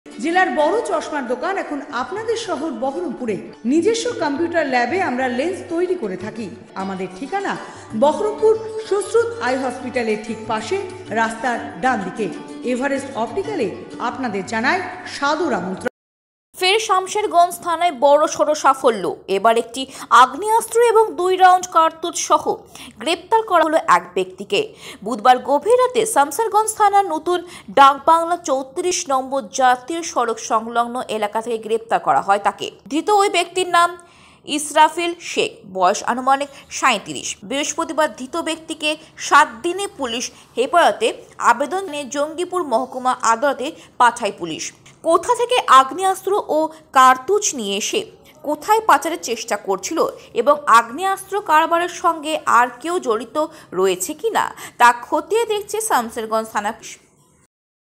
Samserganj Arms: একটি আগ্নেয়াস্ত্র এবং দুই রাউন্ড কার্তুজ সহ গ্রেপ্তার এক ব্যক্তি সামশেরগঞ্জে
আগ্নেয়াস্ত্র কারবারের সঙ্গে আর কেউ জড়িত রয়েছে কিনা তা খতিয়ে দেখছে সামসেরগঞ্জ থানার পুলিশ।